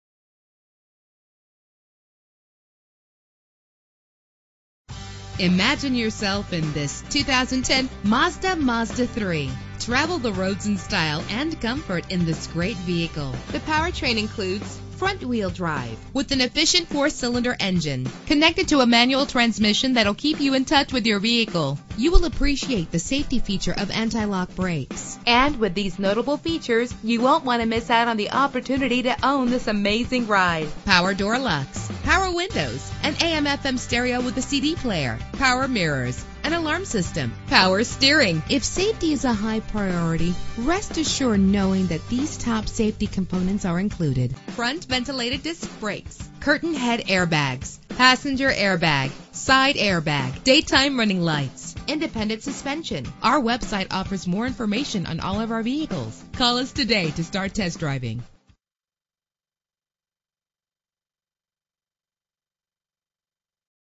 2010 Mazda MAZDA3 - Calgary AB
Phone:
Year: 2010
Make: Mazda
Model: MAZDA3
Trim: GX
Engine: 2.0 liter inline 4 cylinder 16 valve
Transmission: 5-Speed Manual
Color: White
Mileage: 59576
Address:
665 Goddard Ave NE
Calgary, AB T2K 6K1